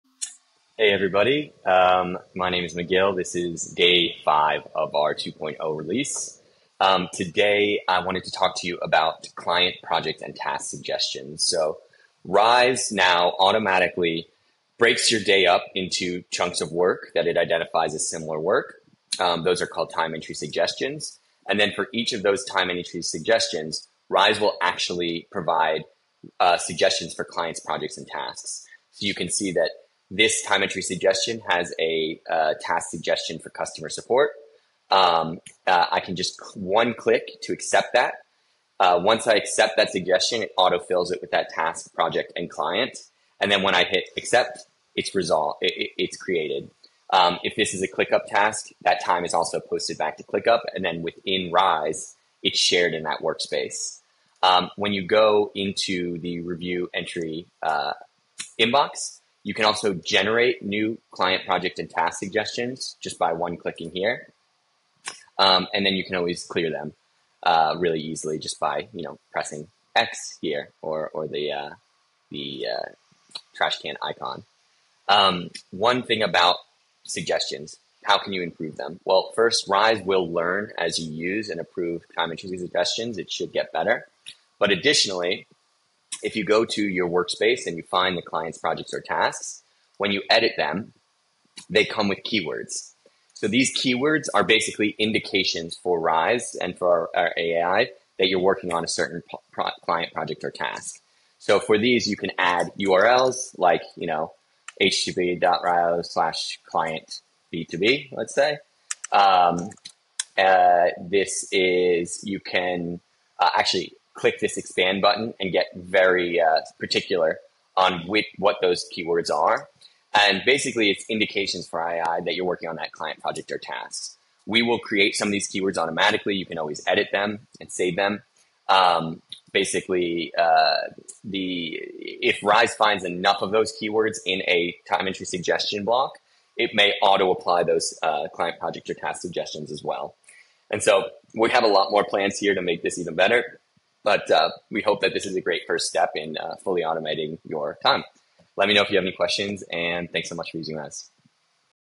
Rize Client, Project, and Task Suggestions 2.0
Rize now understands your work.

In this Day 5 update of the Rize 2.0 launch, we walk through how Rize automatically suggests Clients, Projects, and Tasks for every Time Entry Suggestion — making it even easier to organize your day with zero manual effort.

Here’s what’s new:
✅ Rize now provides Client, Project, and Task suggestions for each Time Entry.
✅ You can add keywords to help Rize’s AI recognize your work automatically.
✅ Rize learns from your accepted entries to improve suggestions over time.
✅ Fully integrates with ClickUp — accepted time entries sync instantly.

This is another step toward fully automated, intelligent time tracking.